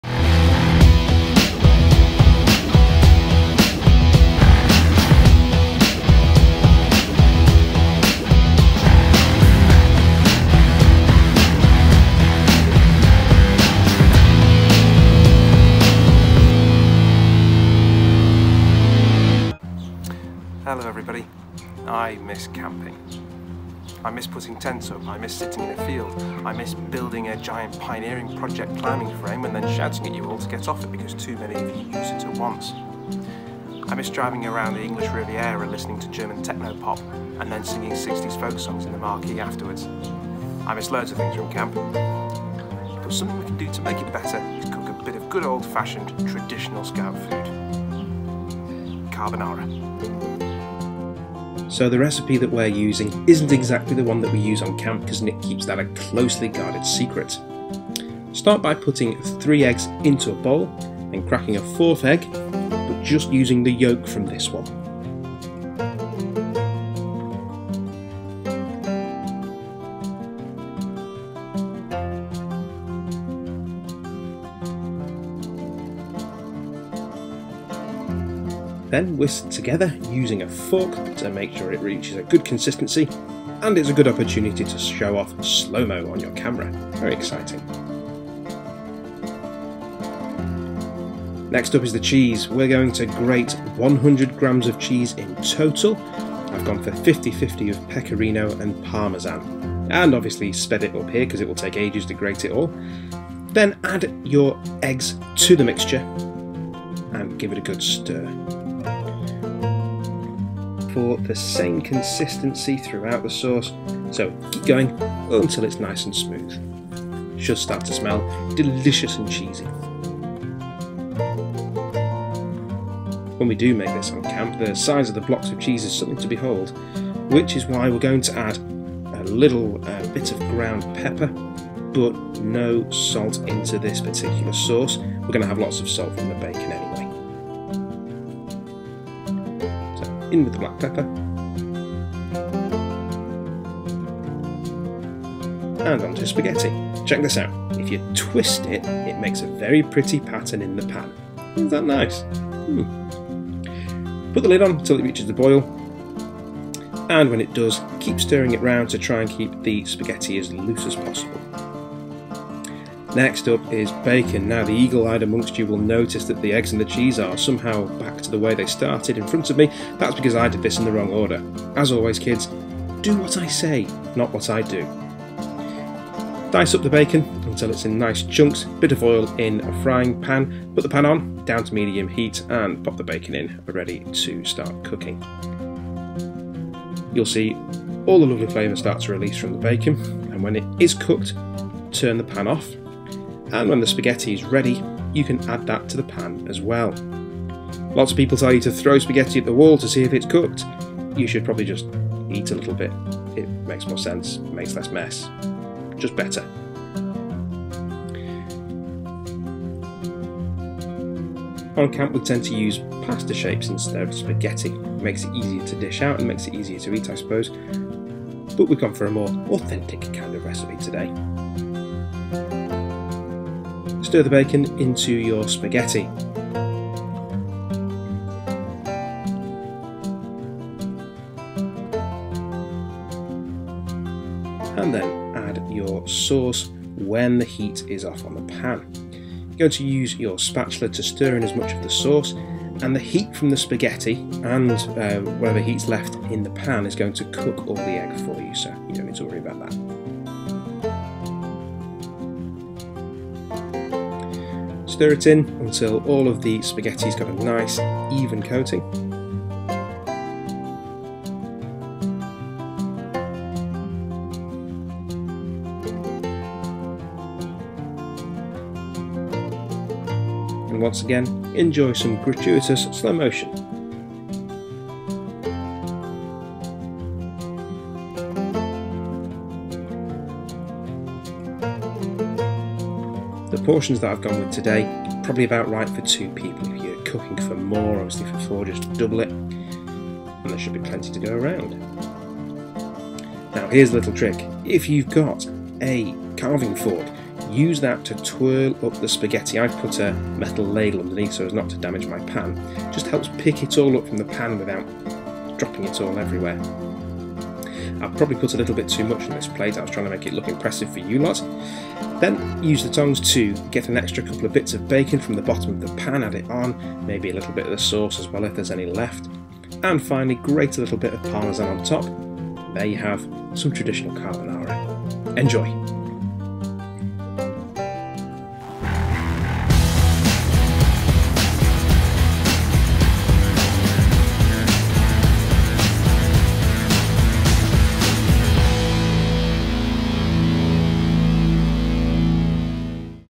Carbonara
It's OK to miss camp - make up for it by cooking the best meal on camp!